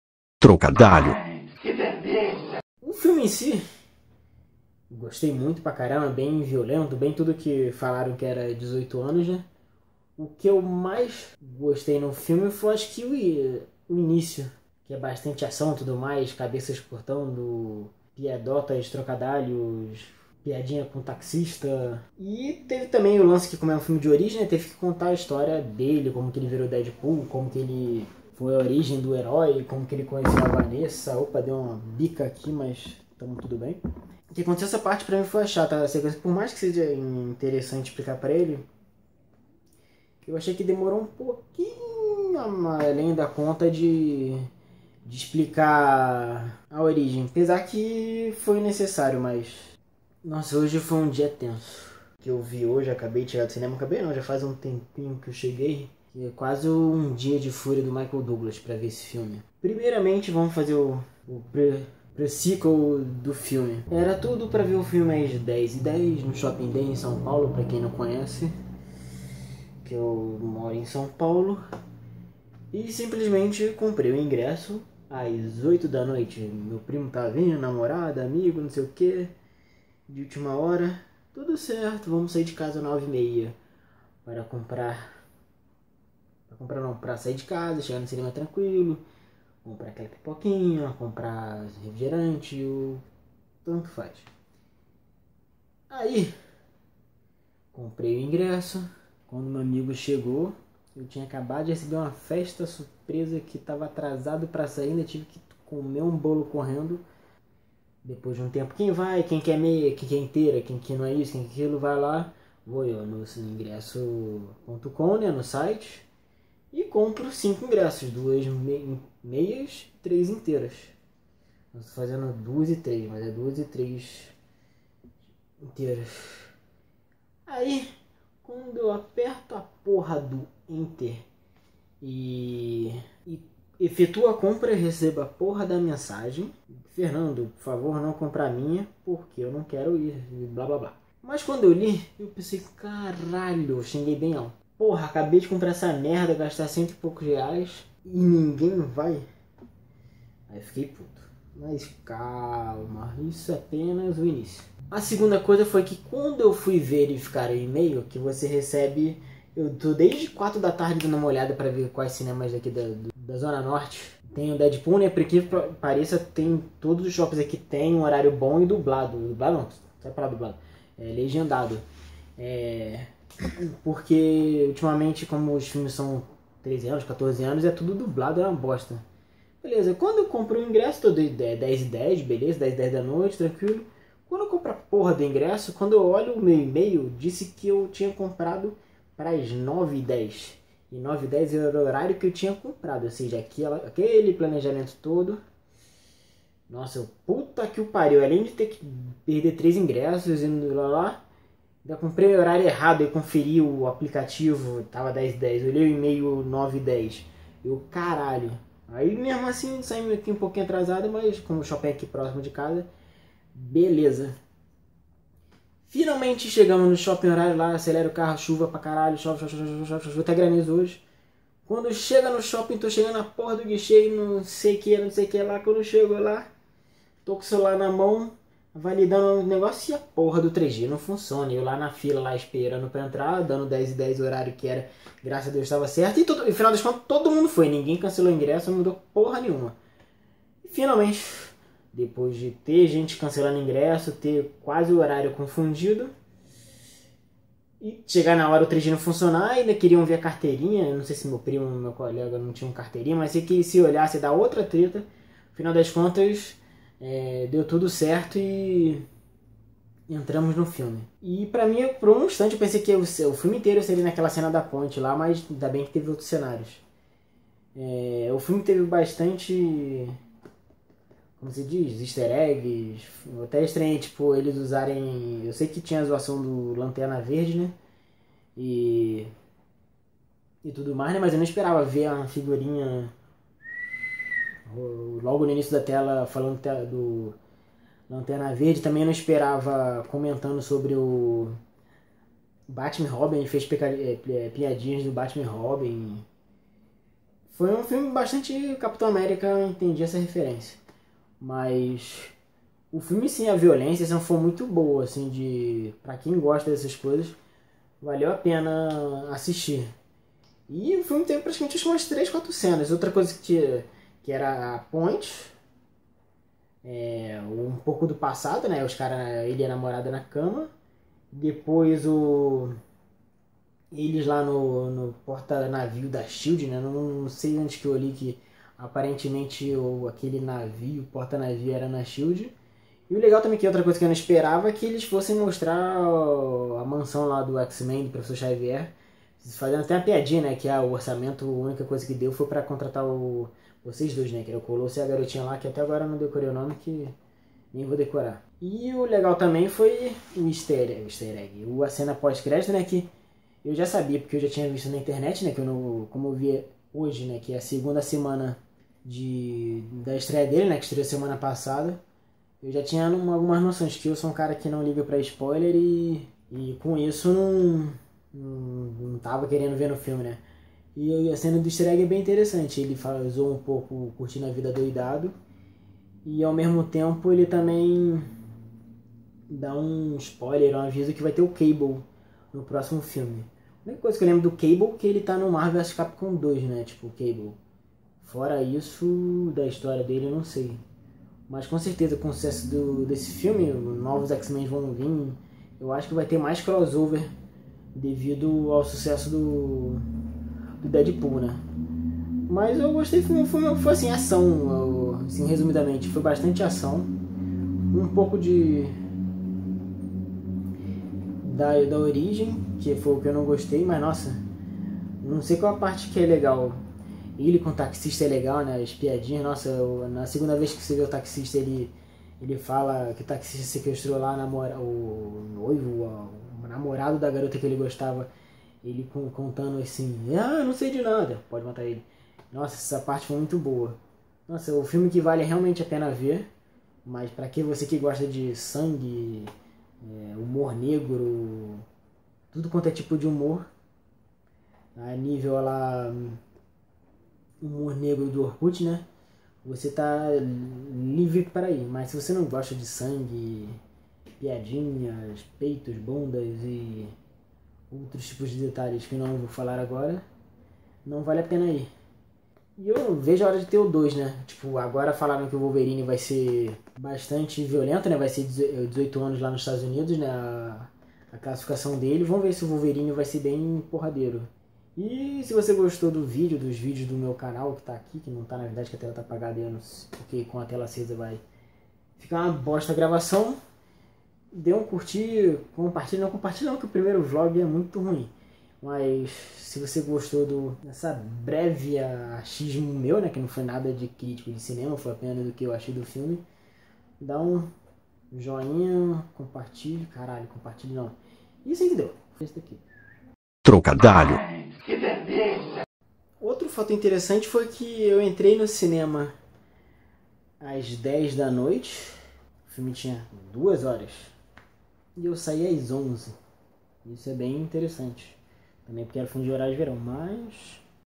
Deadpool (Spoiler Alert) / Um dia de Fúria / Viagem no Tempo
Um breve vlog / achismo sobre o filme do anti-herói da Marvel / Fox.

OBS: Eu percebi que a camera frontal do Galaxy S5 ficou uma b. , mas como era primeiro blog, precisava me ver pra me encontrar no video. kkk

Trocadalho - Humor & Gameplays